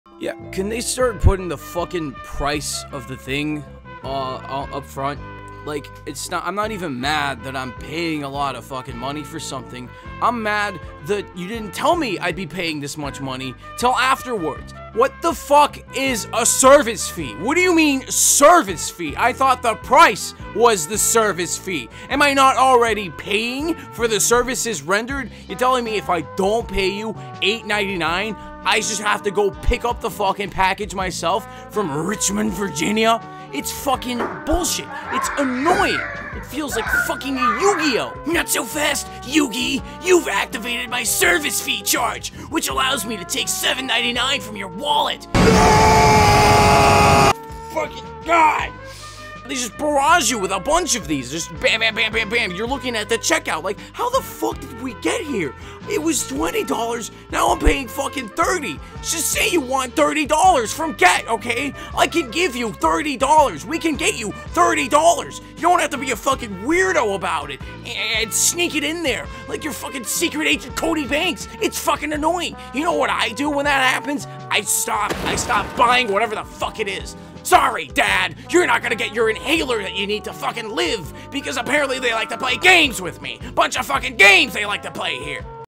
I HATE SERVICE FEES AND HIDDEN FEES
Sometimes I sit in front of a microphone. sometimes im mad when i do it. Every time I hate Service fees